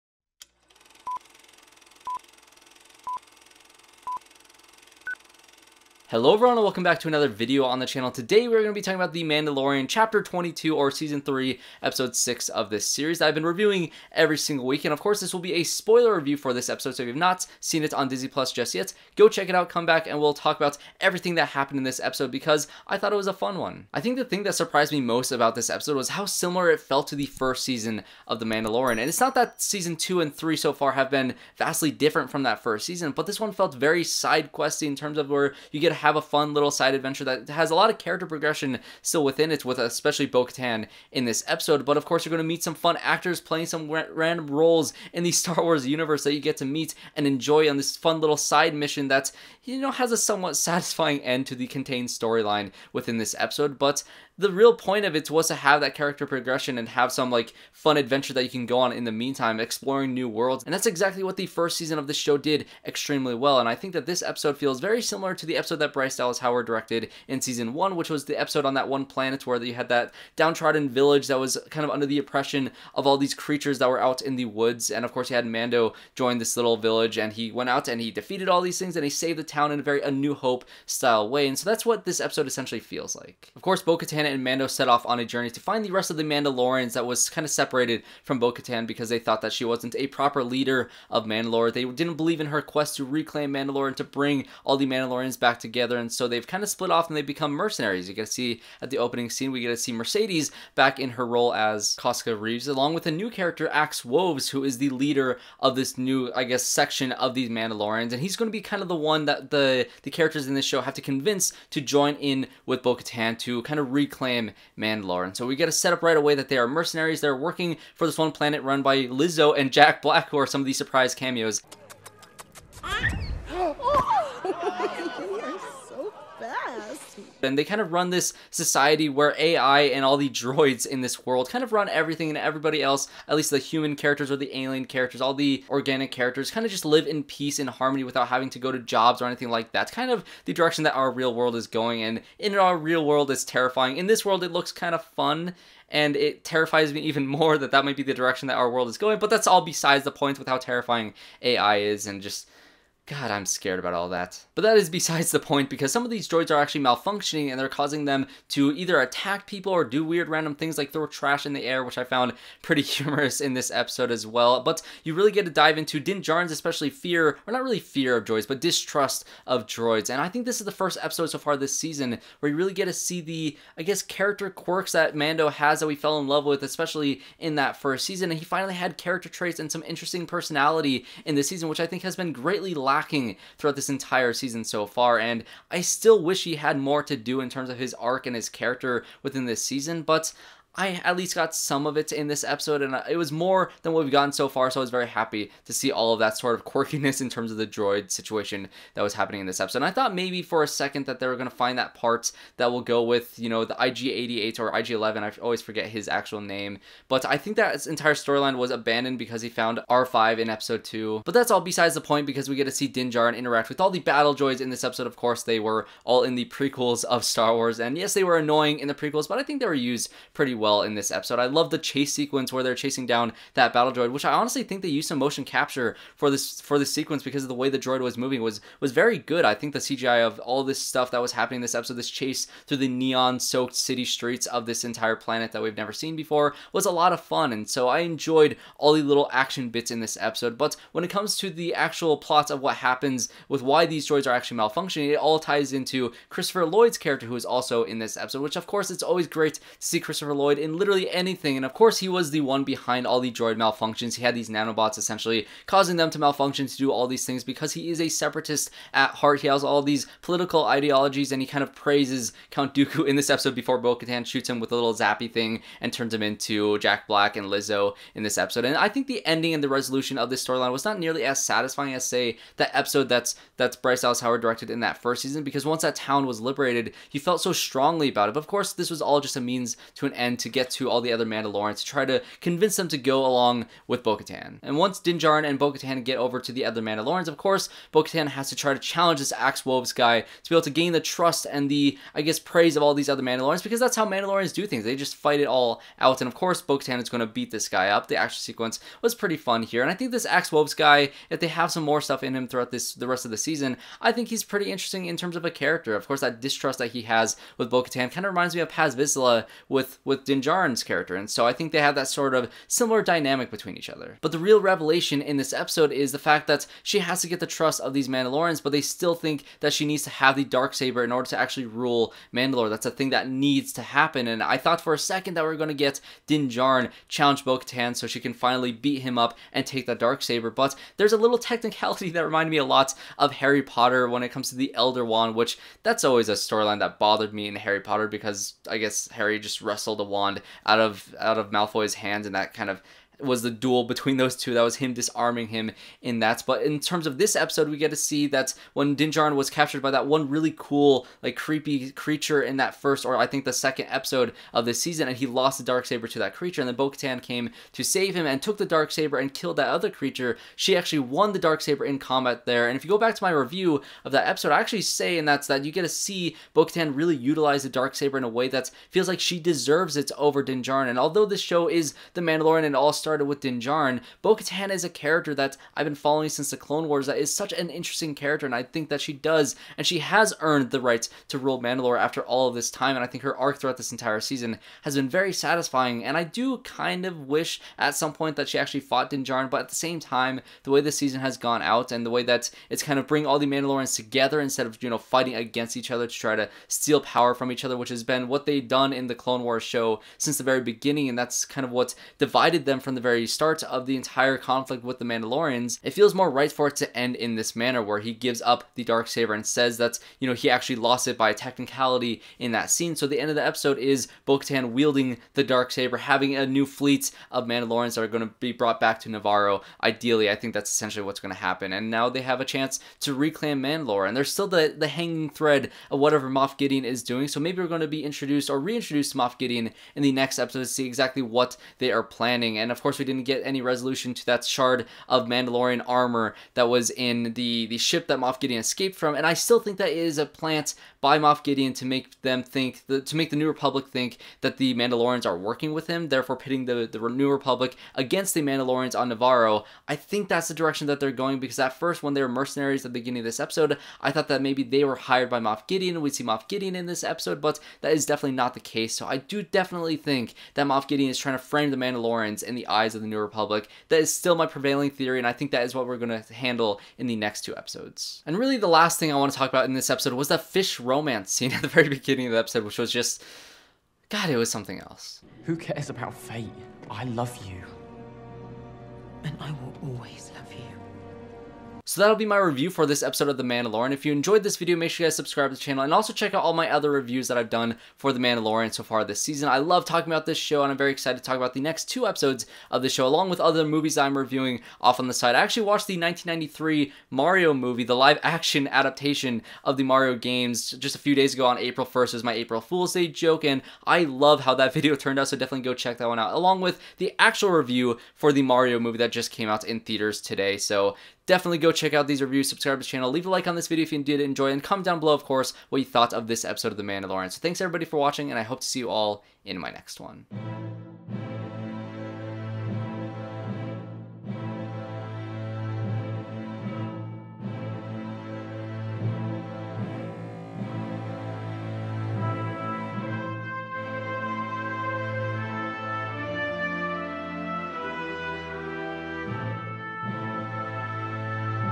The Mandalorian: Chapter 22 - Guns for Hire | SPOILER Review
Are ya ready for a side quest!?!

The Mandalorian Season 3 Episode 6 may take a slight detour in story, however it maintains some great character progression!!!

Starring:
Katee Sackhoff as BO-KATAN KRYZE
Pedro Pascal as THE MANDALORIAN
Mercedes Varnado as KOSKA REEVES
Simon Kassianides as AXE WOVES
Jack Black as CAPTAIN BOMBARDIER???
Lizzo as THE DUTCHESS???
Christopher Lloyd as COMMISSIONER HELGAIT???
Baby Grogu as BABY FREAKING GROGU

Directed by Bryce Dallas Howard

Created by John Favreau and Dave Filoni